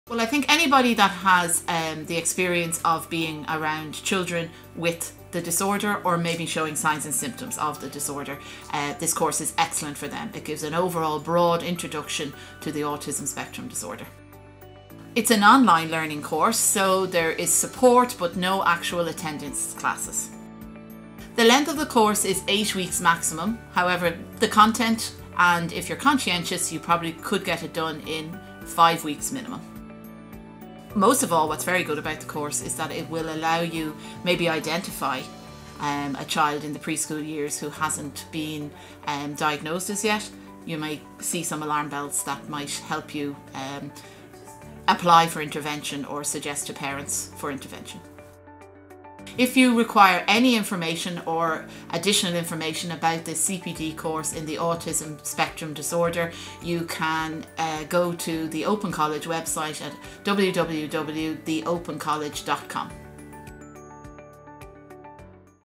Autism Spectrum Disorder in the Early Years Sector - Online Course
We are delighted to announce the launch of our next Childcare & Additional Needs CPD (continuous professional development) short course - Autism Spectrum Disorder in the Early Years Sector.

The purpose of this new and unique Open College designed Autism Disorder Spectrum in the Early Years Sector course is to equip the learner with the necessary knowledge about Autism Spectrum Disorder (ASD) in early childhood.

It will endeavour to provide information about the Autism Spectrum Disorder (ASD), the indications of the disorder within a child and signs and symptoms that are related to the Autism Spectrum Disorder (ASD).

Topics covered include:

• What is Autism?
• History of Autism Spectrum Disorder
• Understanding Autism
• Autism Spectrum Disorder - A Developmental Disorder
• Cognitive Theories of Autism
• How Can Autism Be Managed?